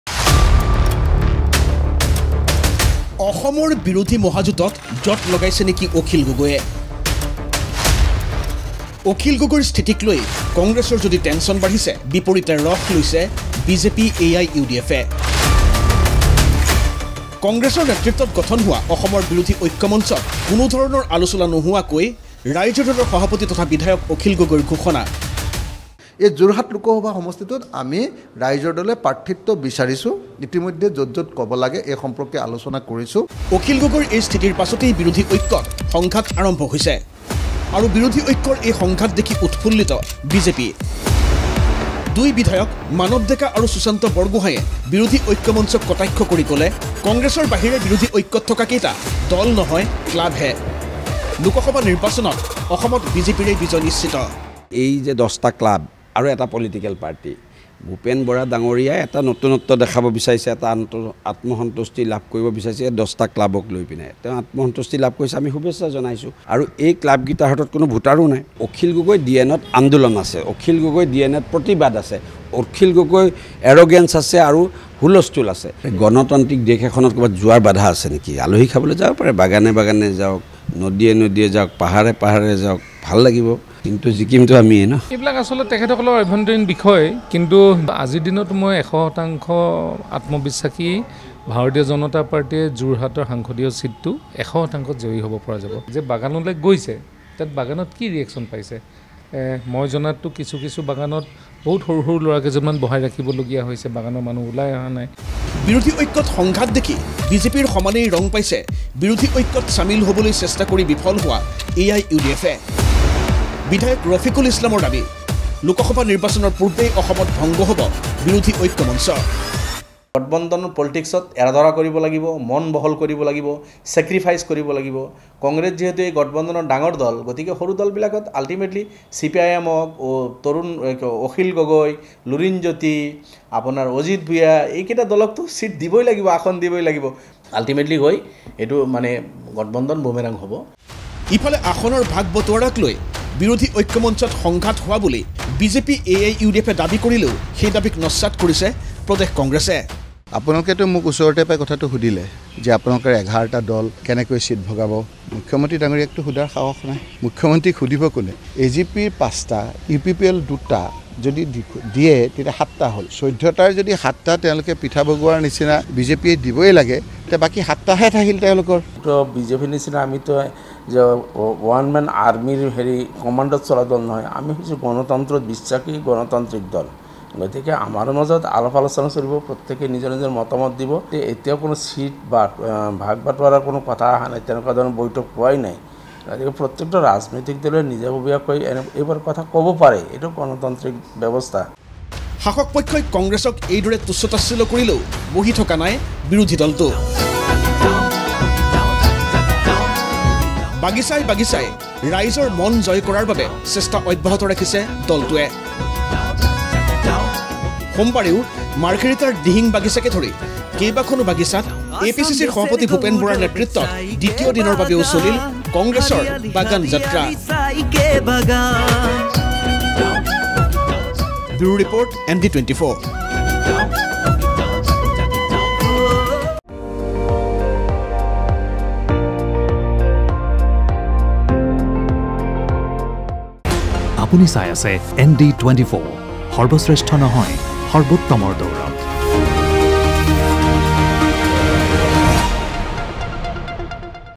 Akhil Gogoi News: অসমৰ বিৰোধী মহাজোঁটত জঁট লগাইছে নেকি অখিল গগৈয়ে?
নিৰ্বাচনৰ পূৰ্বে দুফাল হ'ব নেকি কংগ্ৰেছৰ নেতৃত্বত গঠন হোৱা অসমৰ বিৰোধী ঐক্যমঞ্চ? বিৰোধী মহাজোঁটত জঁট লগাইছে নেকি অখিল গগৈয়ে? ইফালে বিৰোধী ঐক্যত আউল দেখি ৰস পাইছে বিজেপিয়ে।